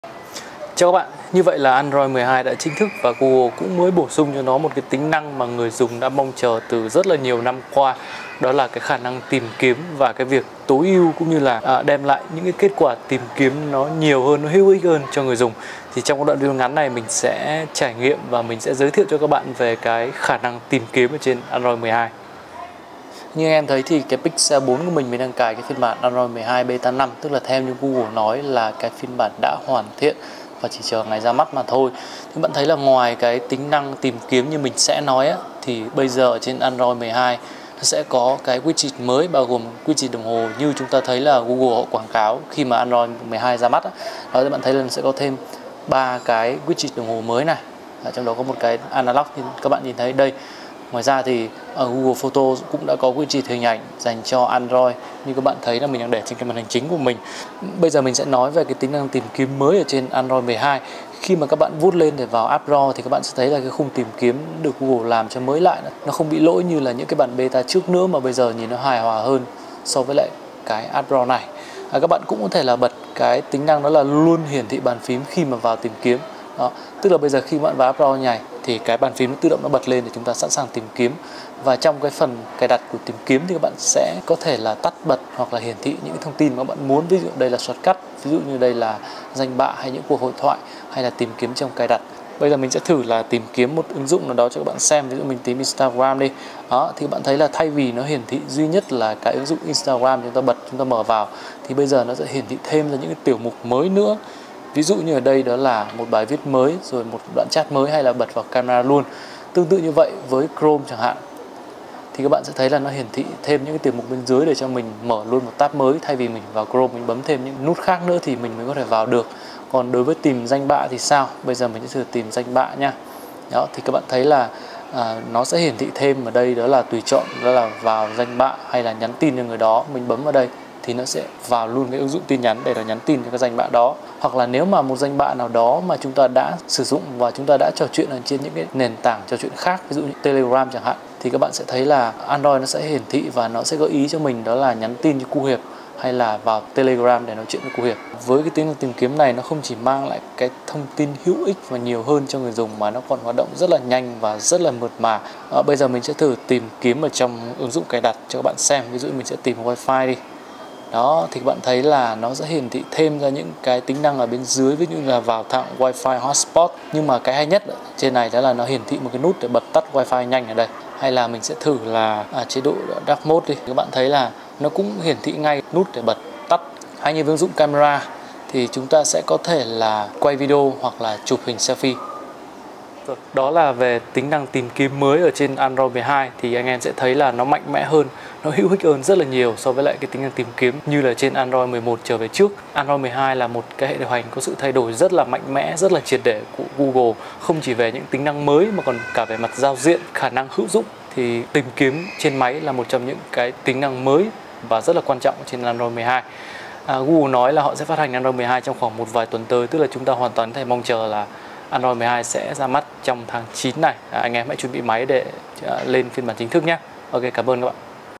Tính năng tìm kiếm mới trên Android 12: rất nhanh và mạnh mẽ
Android 12 beta 5 phát hành hôm nay mang tới một tính năng mới mà người dùng đã mong chờ từ lâu: khả năng tìm kiếm mạnh mẽ và tốc độ. Cái việc tìm kiếm trên máy từ Android 11 trở về trước nó không hoàn thành tốt nhiệm vụ và người dùng chẳng có được nhiều thông tin từ tìm kiếm như trên Android 12.